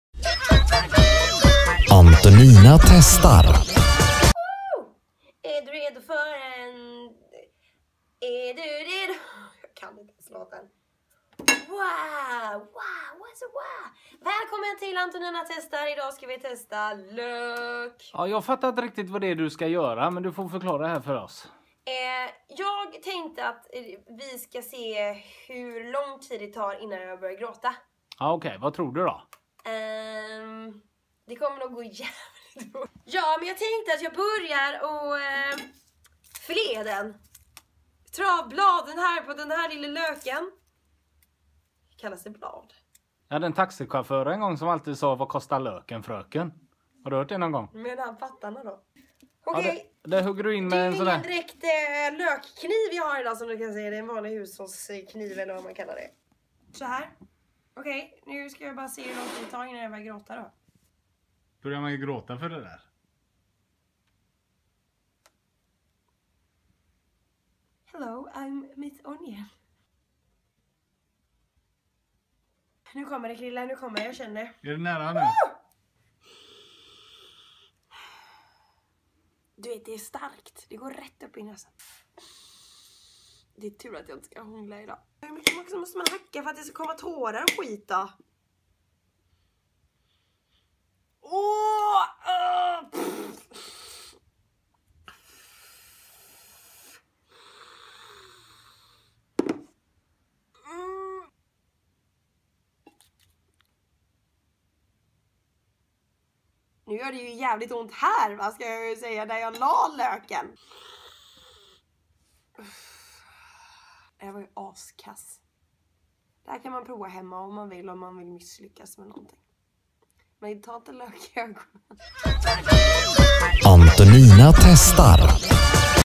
Lök
Idag ska Antonina börja gråta med hjälp av en gullök, man börjar ju alltid grina när man hackar lök säger Nina.